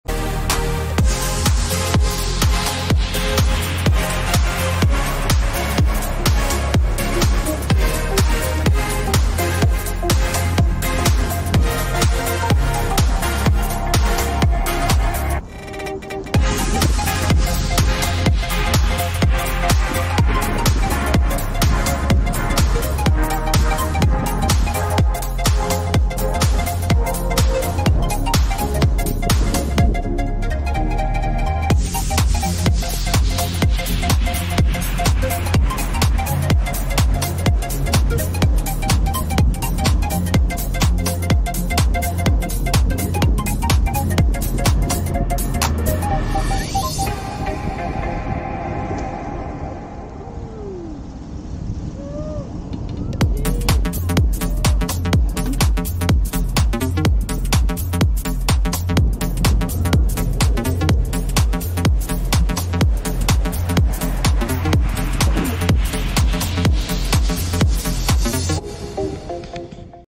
Surf Skate Crave
Just trying out a small downhill slope with my Surf Skate.